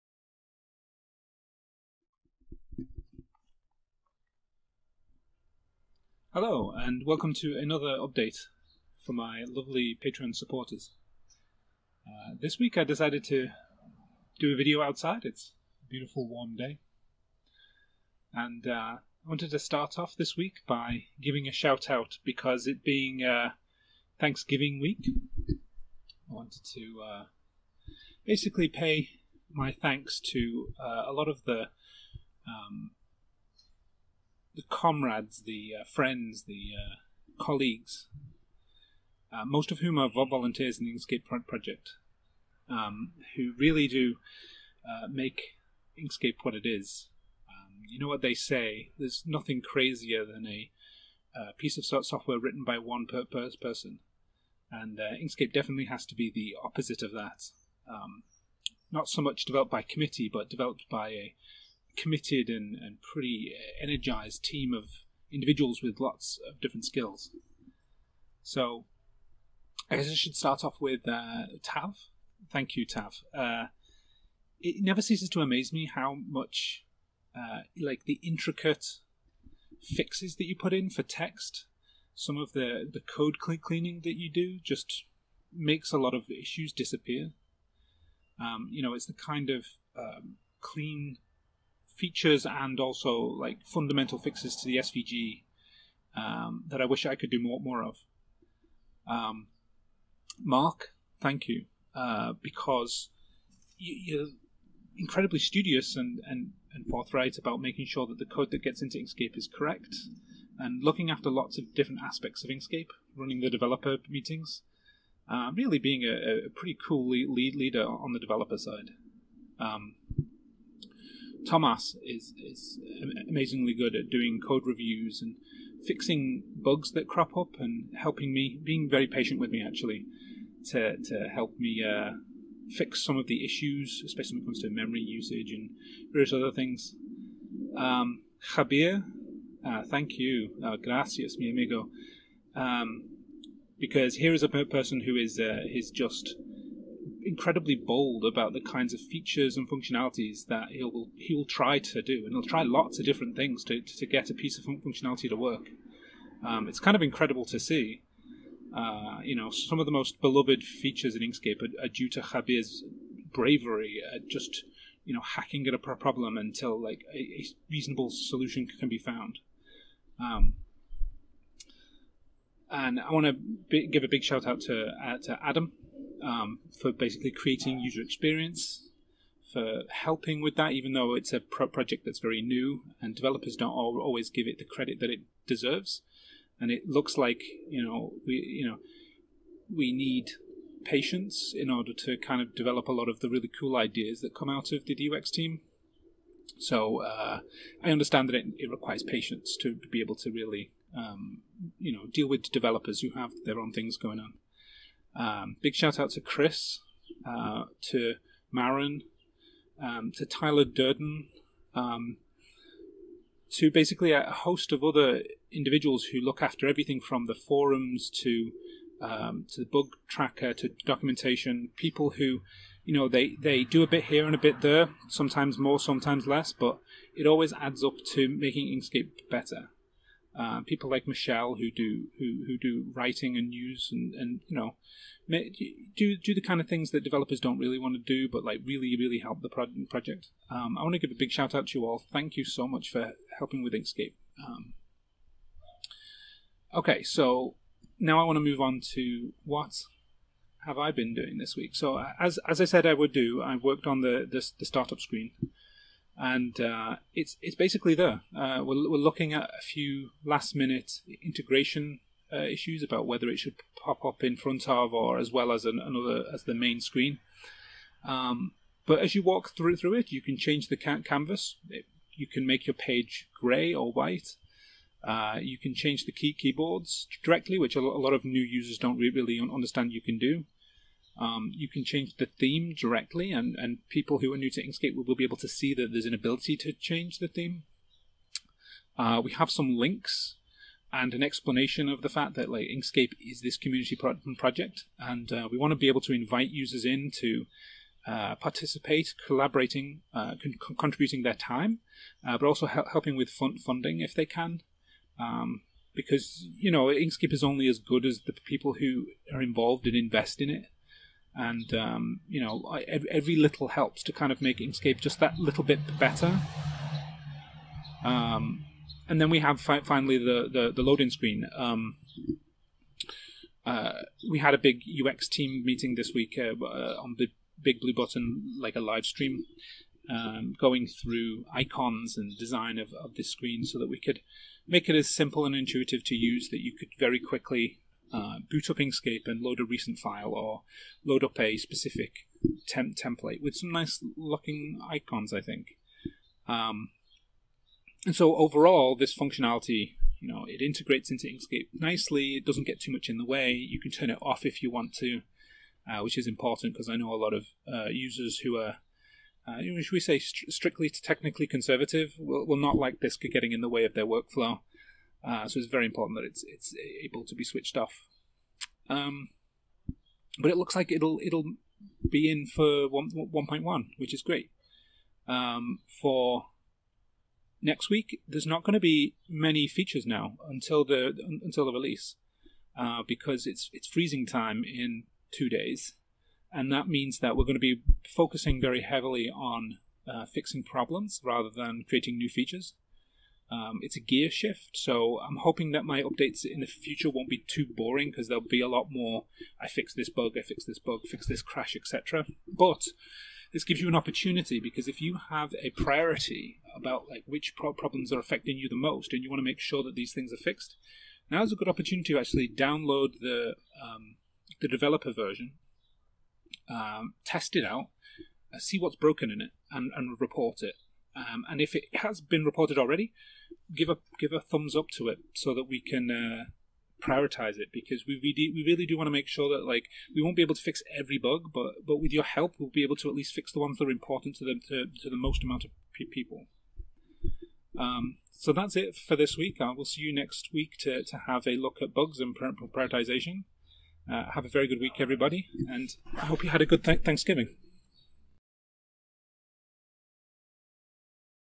Startup screen thanks giving special - Inkscape developer update Nov 28th 2020
This week I show the finished startup screen as well as give my thanks to the many great people in the Inkscape project.

0:00 - Welcome
0:26 - Give Thanks

4:07 - Startup screen
6:18 - Freezing Times ahead!